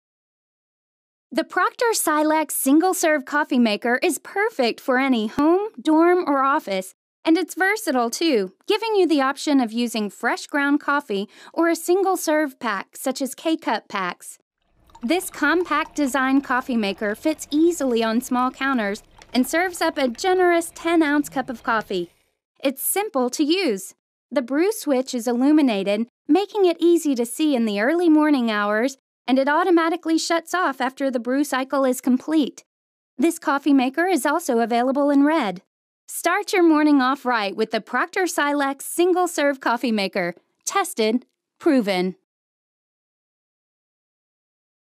Proctor Silex Single Serve Coffee Maker | Blain's Farm & Fleet
Start off your day with a refreshing cup of coffee made in the Proctor Silex Single Serve Coffee Maker. This petite coffeemaker turns off automatically when brew cycle is completed and comes with a bright brew switch.

-Use single-serve packs or ground coffee
-Compatible with any K-Cup pack
-10 oz. capacity
-Compact design
-Auto shutoff after brew cycle complete
-Illuminated brew switch
-Mug not included